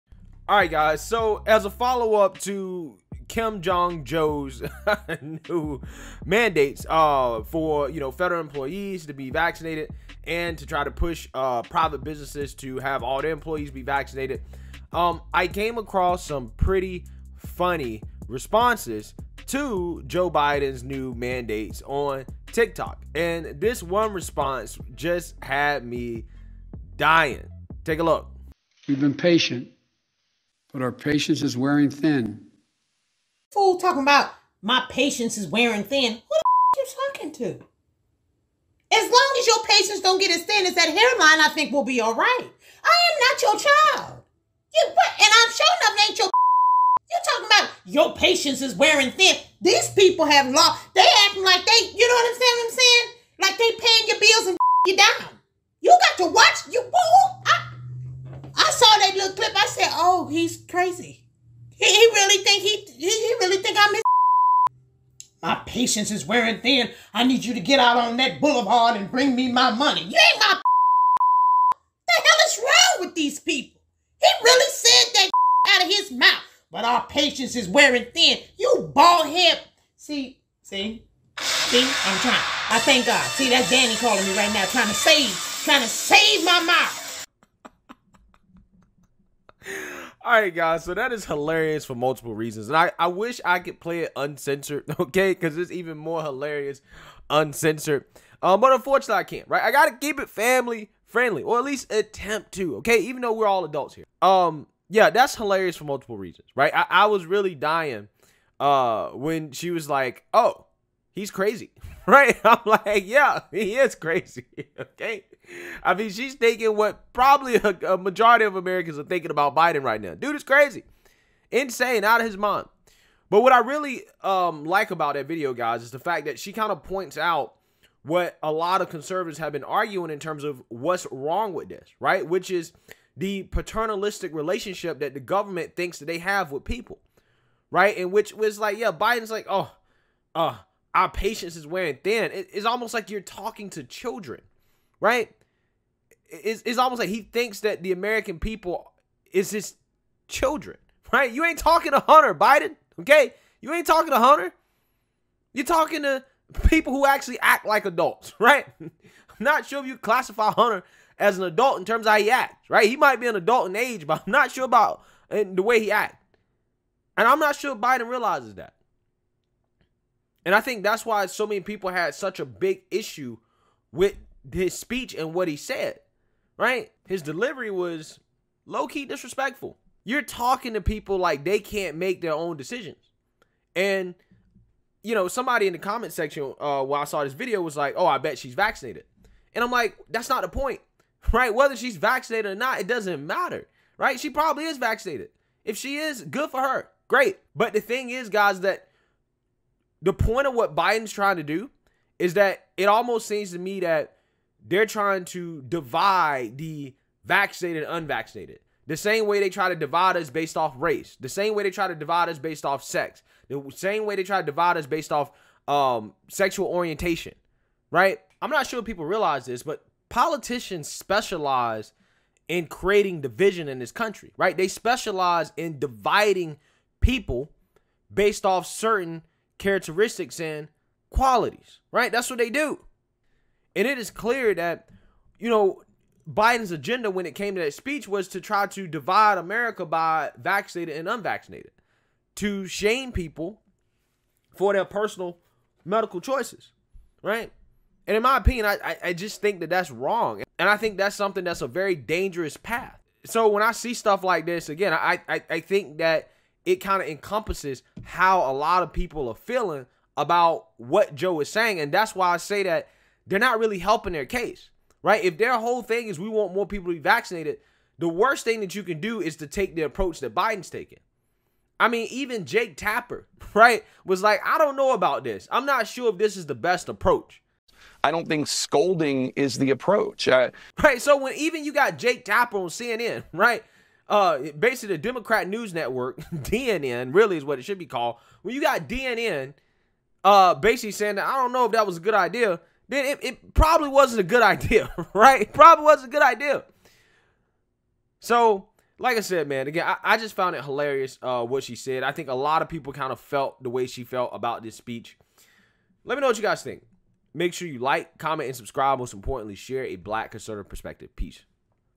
Black Woman Has EPIC CLAP BACK Response To Joe Biden's Mandate Speech
TikToker meganmcglover has an epic response to Joe Biden's mandate speech.